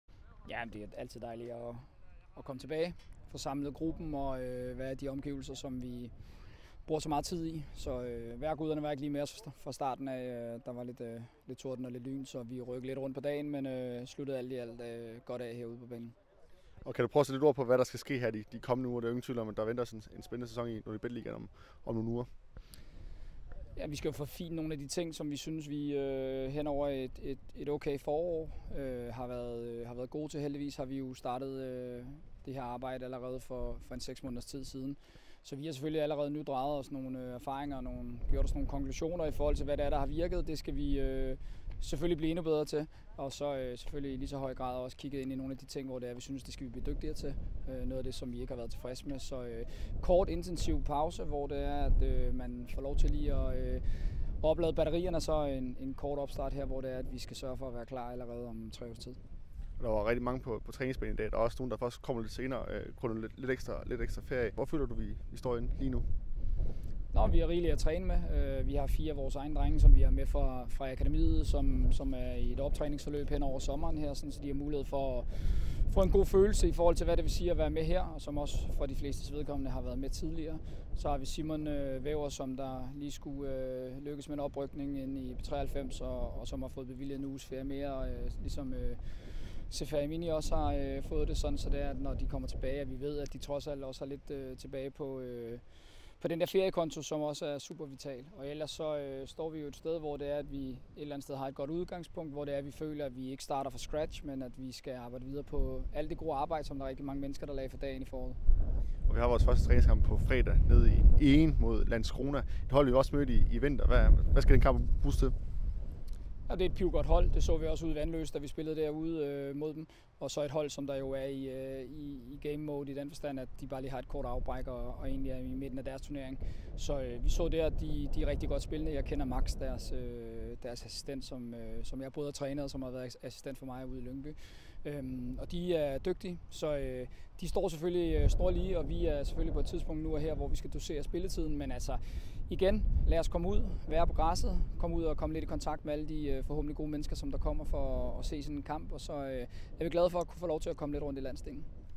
Nørgaard om første træning
Mandag var spillerne tilbage på træningsbanen, og cheftræner Thomas Nørgaard satte efterfølgende ord på, hvad opstarten skal bruges på, inden sæsonen starter om mindre end en måned.